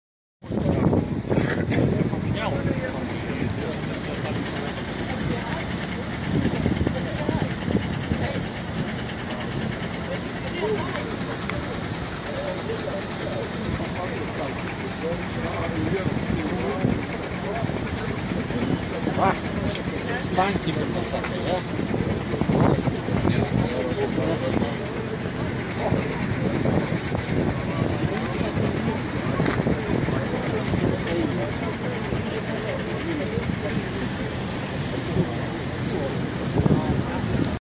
06.12.2019, РИГА, ЛАТВИЯ, Акция у посольства РФ в Латвии (ЧАСТЬ 3)
Акция у посольства РФ в Латвии (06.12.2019 Рига 16-20)